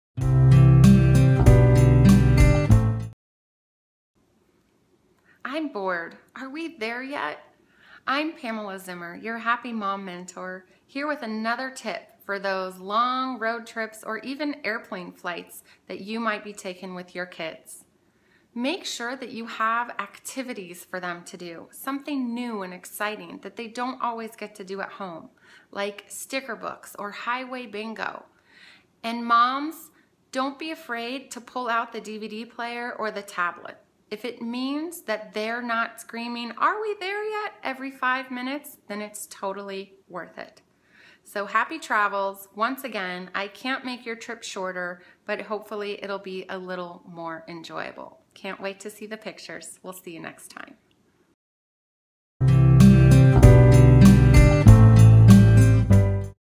HappyMommyMoment 79: Travel Tips
Weekly video inspirations and tips from Pamela Zimmer, Happy Mommy Mentor.
This week: Travel Tips
Description: How often do you hear, “Are we there yet?” or “I’m bored!” Today, I have some tips for a more enjoyable travel for the next time you set out on a family trip.